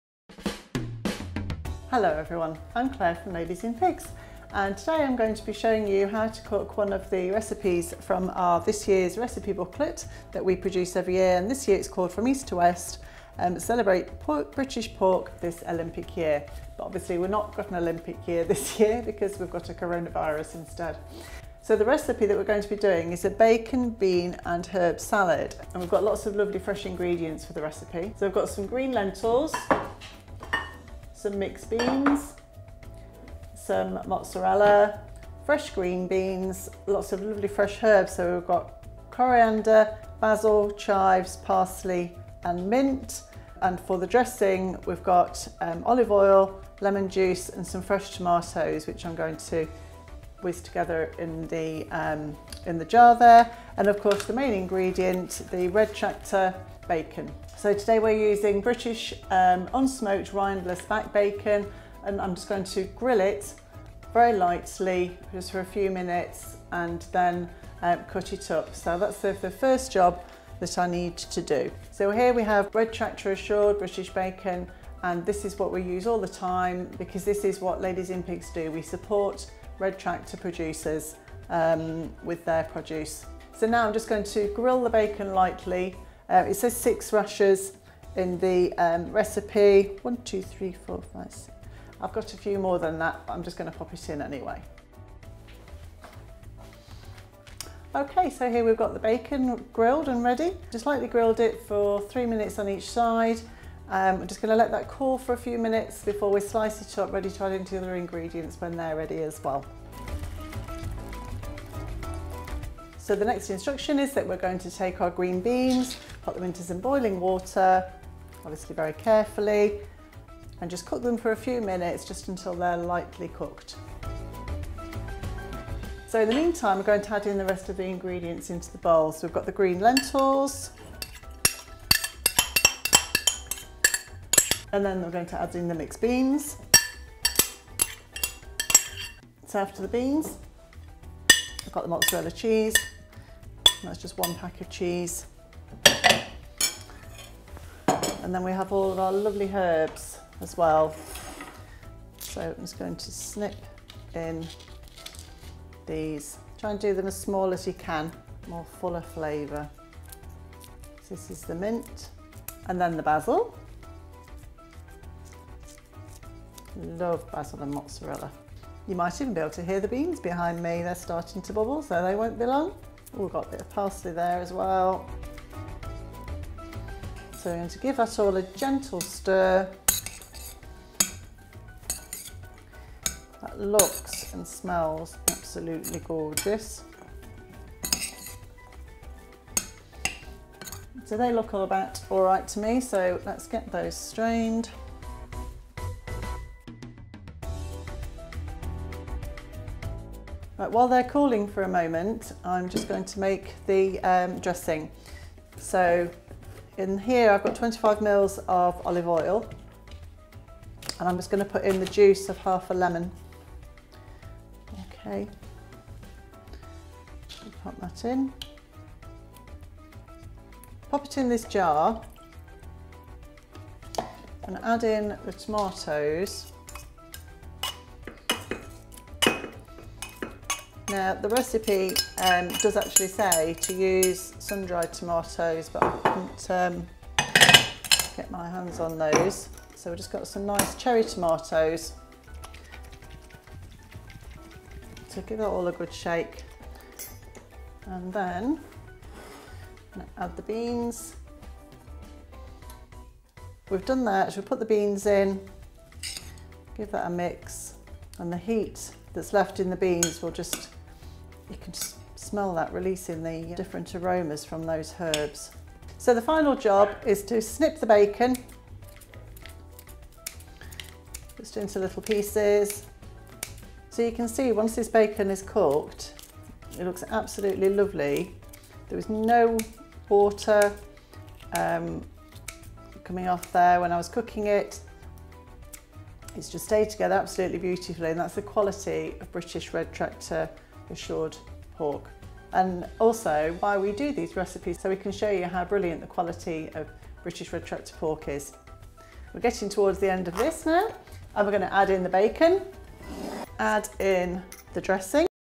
Ladies in Pigs Bacon, bean and herb salad! Perfect for summer!
Here is our quick and easy salad recipe for those sunny days!  Claire is putting together a nutritious, quick and simple dish.

To find the recipe and the rest of the 2020 East to West range please visit: